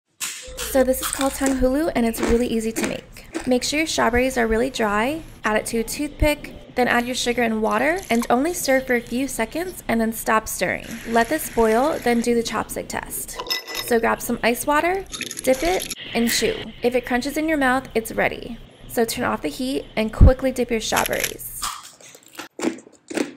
3 ingredient tanghulu 🍓🍓
tanghulu 🍓🍓 ~

These candy coated strawberries are a popular street food in Asia. It’s soo easy to make and requires just 3 ingredients – sugar, water and fruit!

link to ingredients